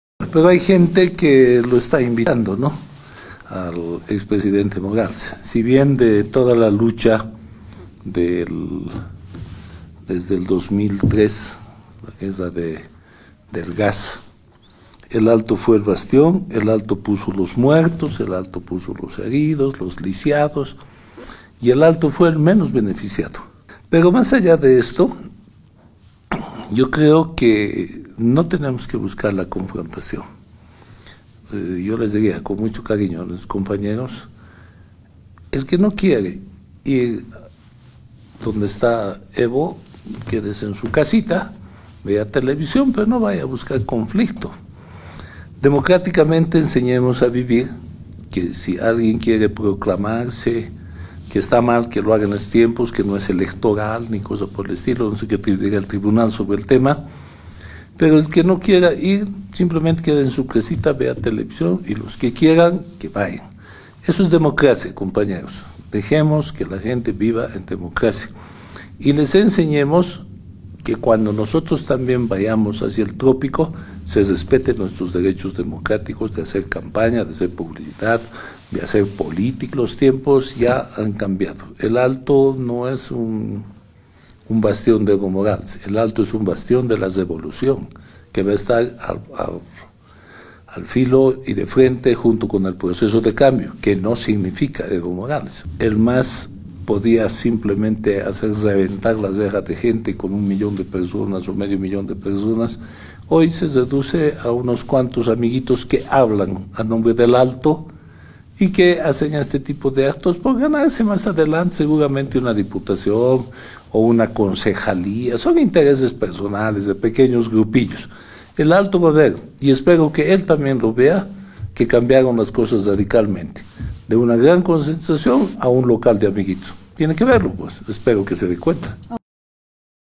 Viceministro Torrico rechaza publicación de The Washington Post; "a los gringos no les creo nada so
Viceministro Torrico rechaza publicación de The Washington Post; "a los gringos no les creo nada sobre todo en drogas”

El viceministro de coordinación gubernamental Gustavo Torrico, afirma que todo lo que viene de los "gringos no es creíble", en respuesta a la publicación de The Washington Post donde asevera que Sebastián Marset sacaba toneladas de droga del Chapare, sin embargo, dijo que nadie va negar que en el Chapare hay droga, pero no en las cantidades que ellos señalan por lo que cuestiona que si tenían esa información por qué no hicieron operativos en las fronteras donde se encuentra la DEA, y considera que lo único que buscan es desprestigiar al país.